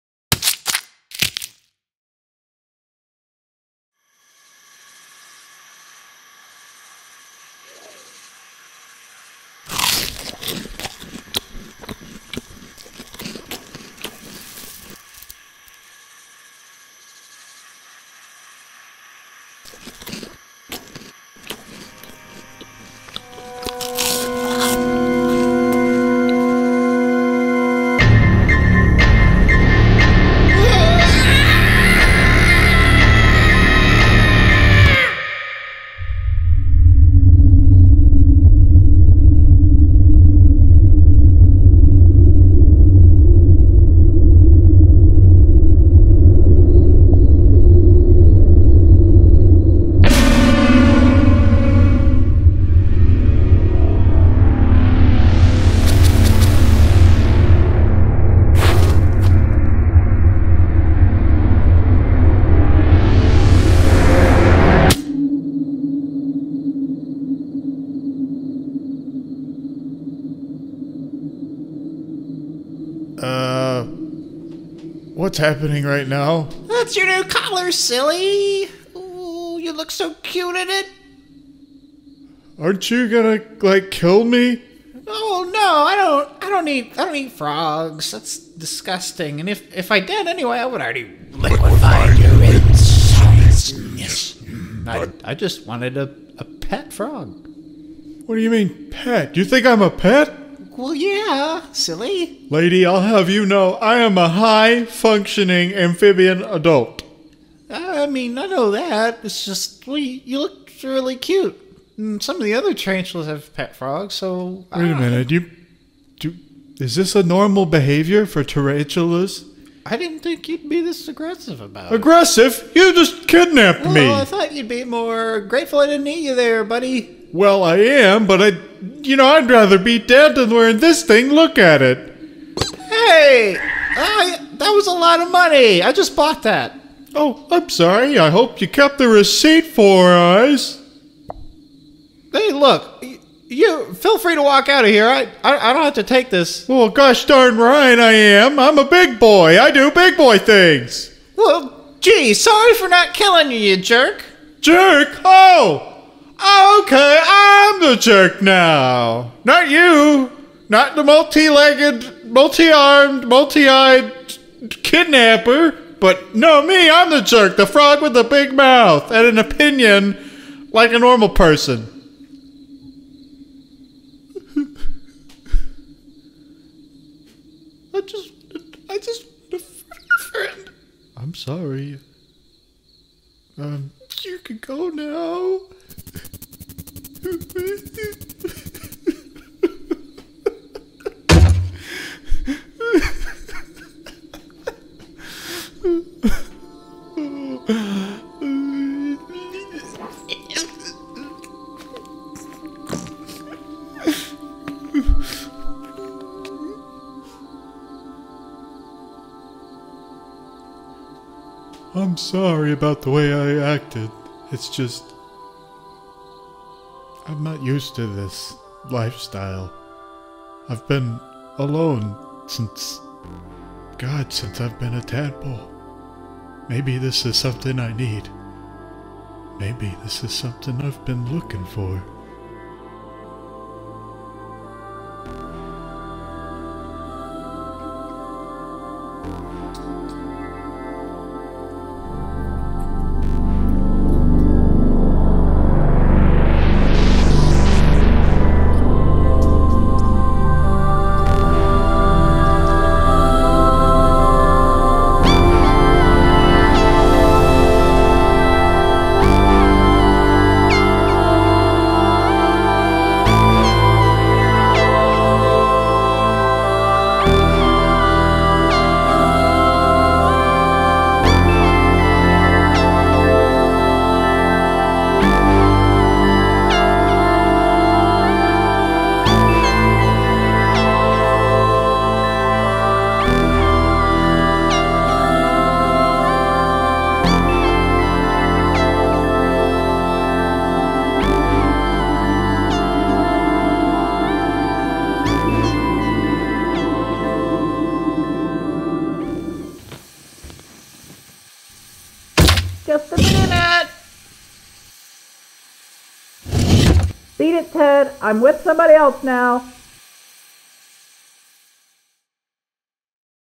Amphibia
When a frog is captured by a large spider, things go from scary to weird real quick.

Written, animated & edited by myself.

Audio by myself and extra sfx pulled from Adobe's Sound Library.

Stay after the credits for a secret stinger!